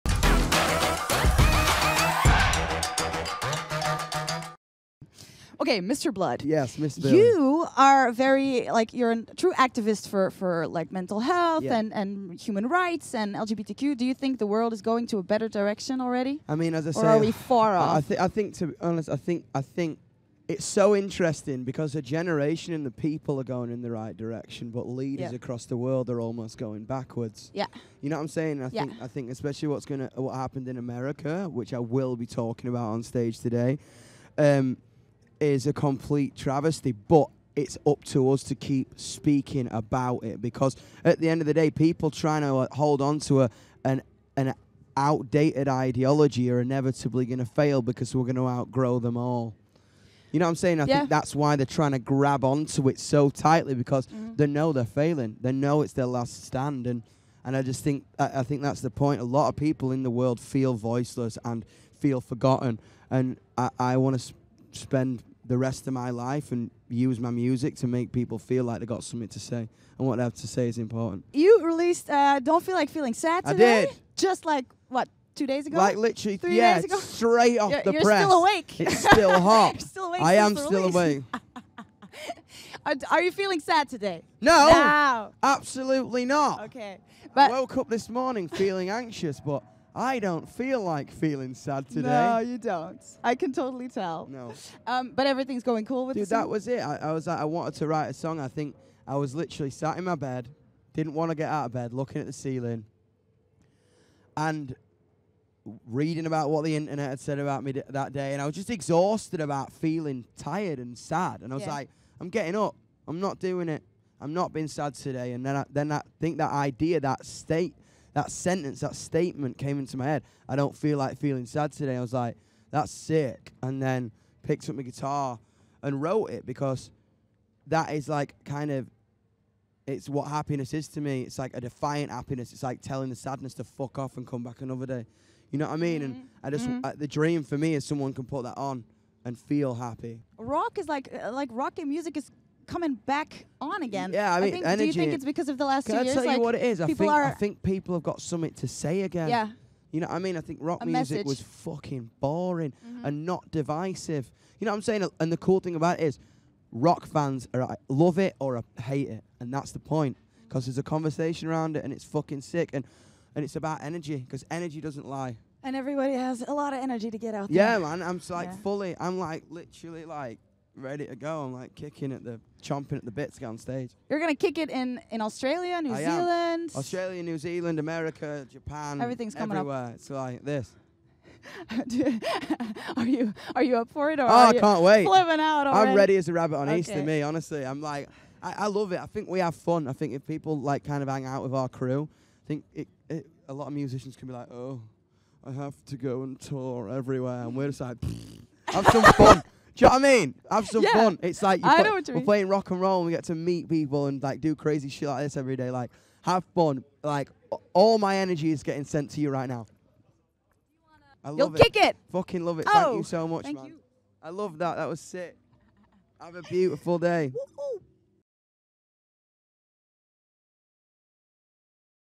RWTV: Interview with Yungblud
Interview with @yungblud at Rock Werchter 2022 on Saturday 2 July.